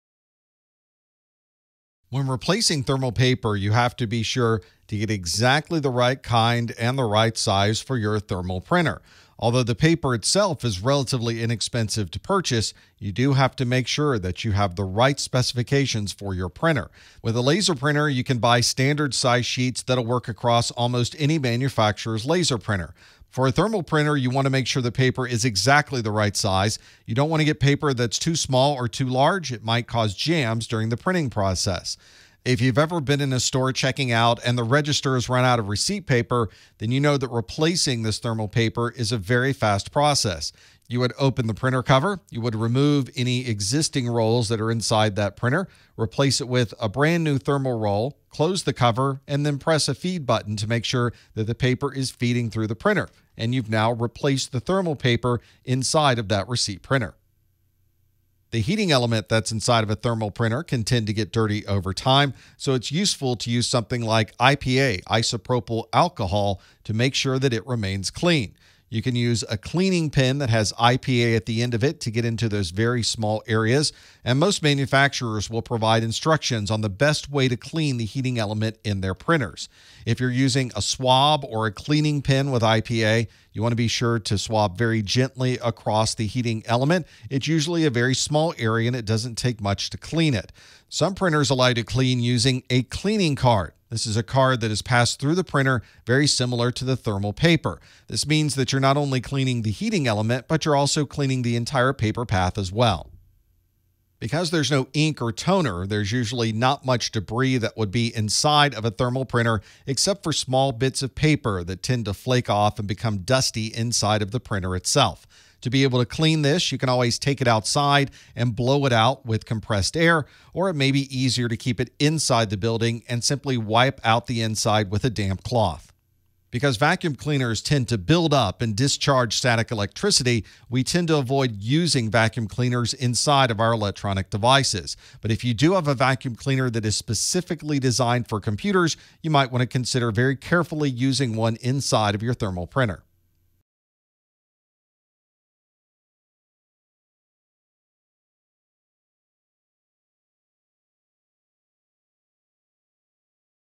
Thermal Printer Maintenance - CompTIA A+ 220-1001 - 3.11
The process of maintaining a thermal printer is relatively easy compared to other printer types. In this video, you’ll learn about replacing thermal paper, cleaning the heating element, and removing debris.